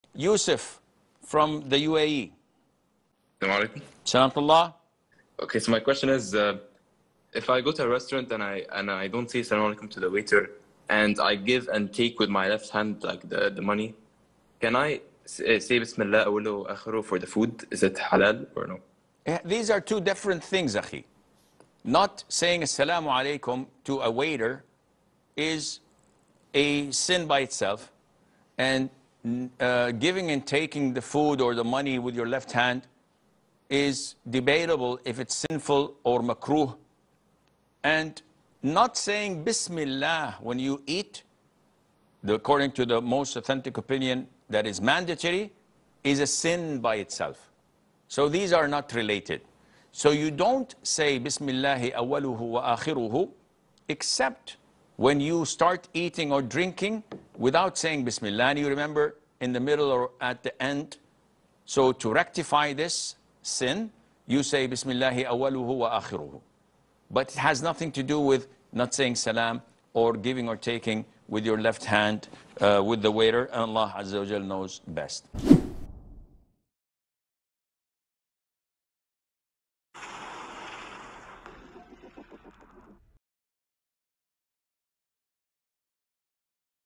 Can I say Bismillahi Fi Awwalihi WA Aakhirihi if I didn’t give Salam & gave money with left hand?
[Workflow]
0:00 Intro
0:05 Question
0:17 Answer
1:15 Outro